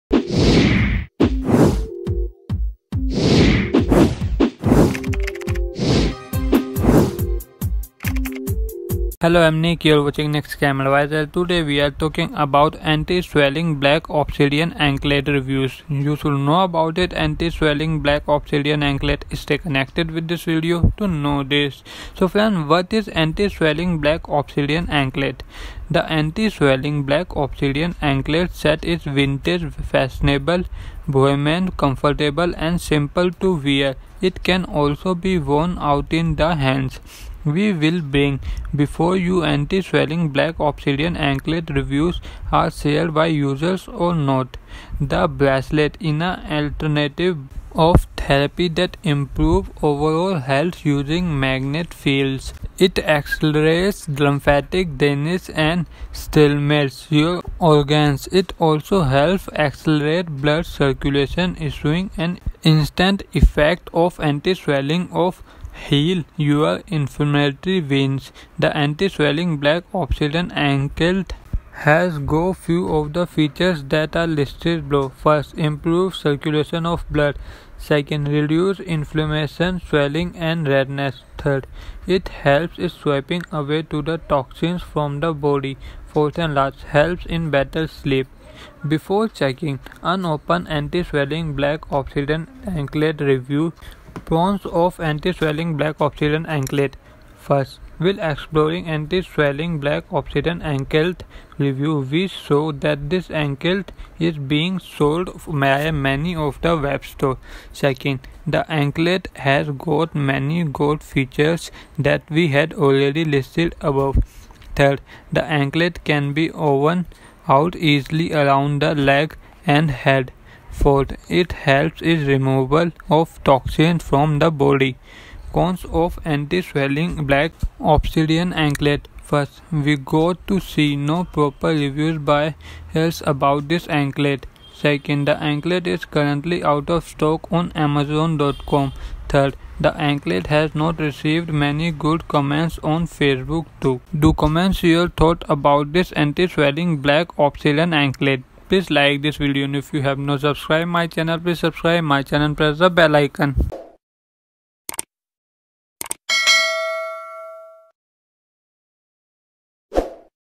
Anti Swelling Black Obsidian Ankle ! Anti Swelling Black Obsidian Ankle Reviews - Watch Full Details
What Is Anti Swelling Black Obsidian Anklet?

The Anti-Swelling Black Obsidian Anklet set is vintage, stylish, bohemian, pleasing, and simple to wear. It can moreover be depleted in the hands.

We will bring before you if Anti Swelling Black Obsidian Anklet Reviews are shared by customers or not.

The arm band is an elective kind of treatment that improves overall prosperity using magnet fields. It speeds up lymphatic waste and stimulates your organs. It moreover accelerates blood course, giving a second effect of Anti Swelling to recover your blazing veins.

The Anti-Swelling Black Obsidian Anklet has very few of the features that are recorded underneath :-

1.Improves stream of blood

2.Reduces exacerbation, growing and redness

3.It helpers in cleaning away the toxins from the body

4.Helps in better rest

Before checking upon Anti Swelling Black Obsidian Anklet Reviews.